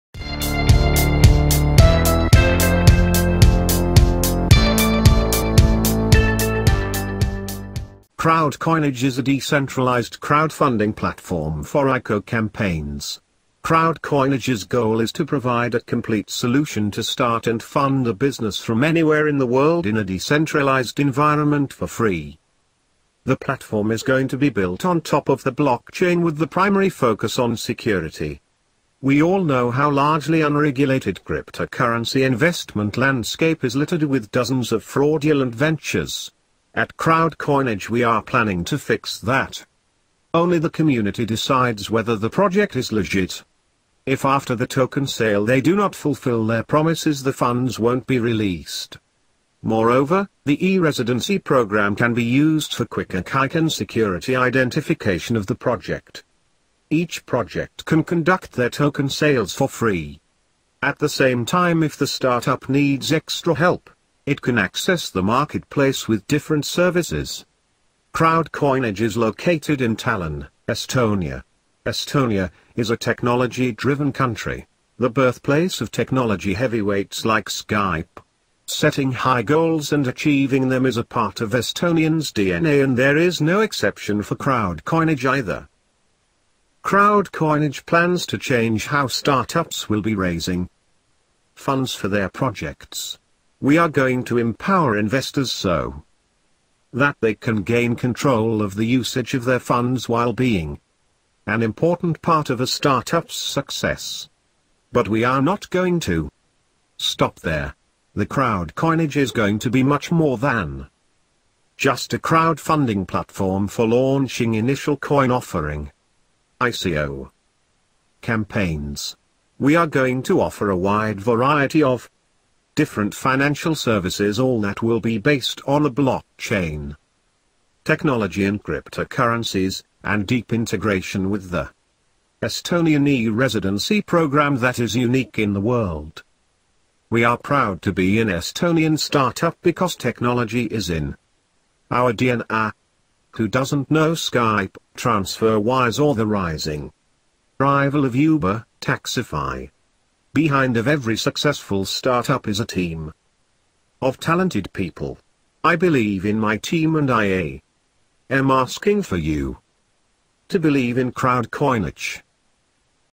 CrowdCoinage : The first and only solution to start bussines
CrowdCoinage is a decentralized crowdfunding platform for ICO Campaigns. CrowdCoinage’s goal is to provide a complete solution to start and fund a business from anywhere in the world in a decentralized environment for free.